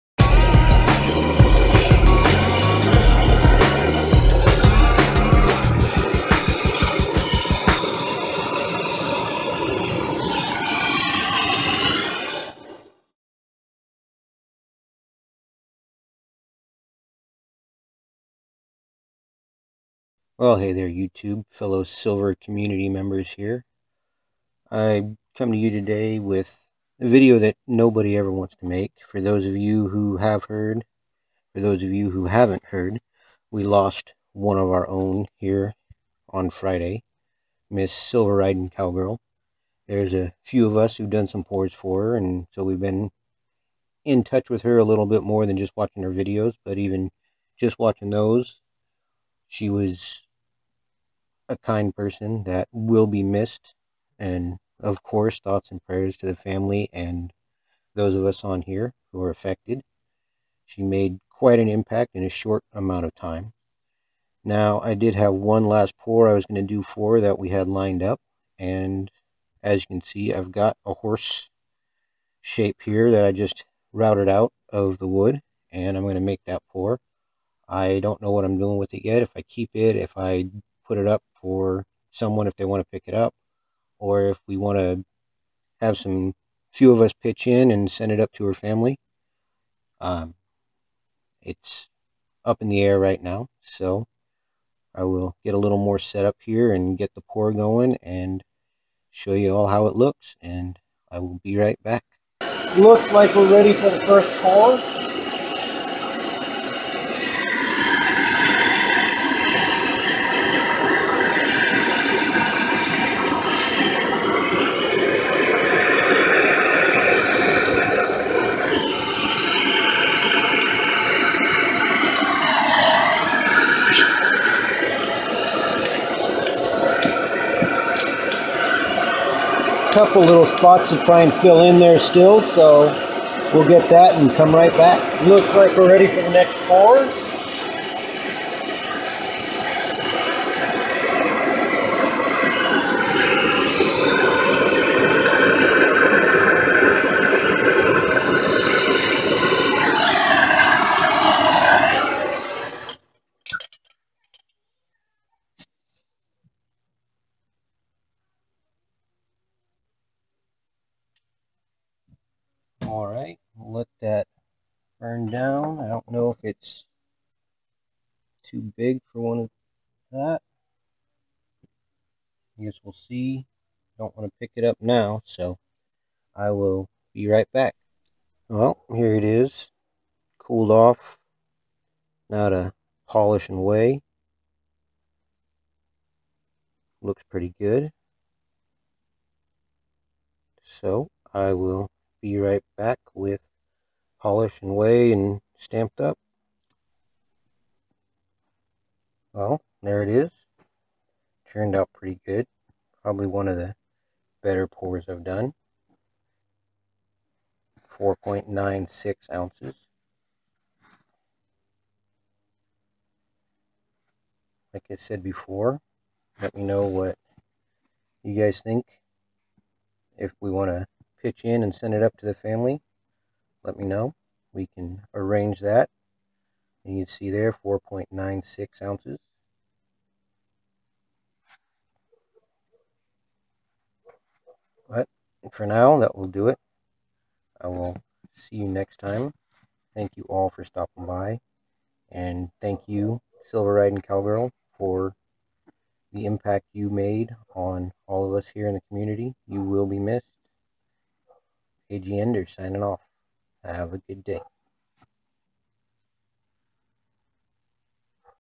So Long Silver Ridin' CowGirl
Rest in Peace SRC.
You made quite an impact on the silver community here on youtube in your shot time, you will be missed.